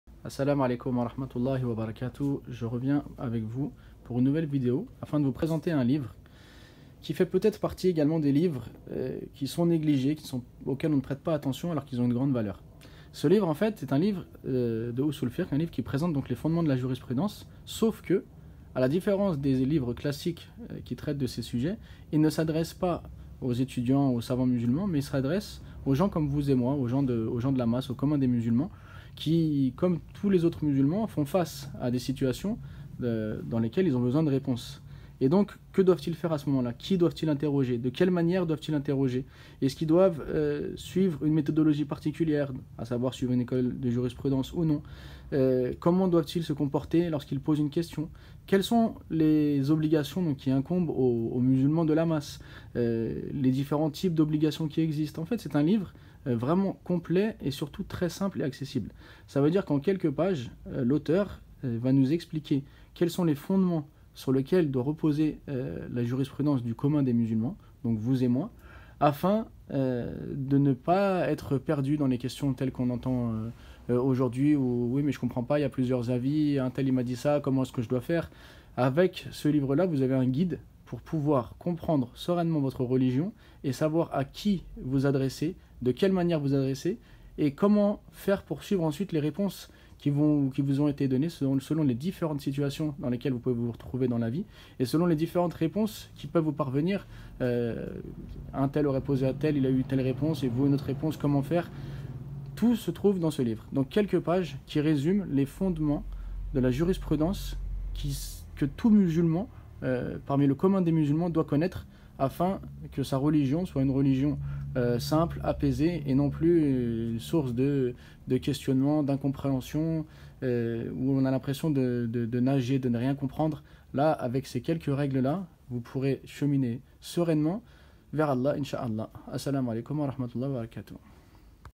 Les règles de fiqh et ses fondements
Voici un petit livre, grand par son contenu, qui expose ce que tout musulman et toute musulmane doit connaître concernant les fondements de la religion.
Ce livre est un véritable manuel pour une bonne compréhension de la religion.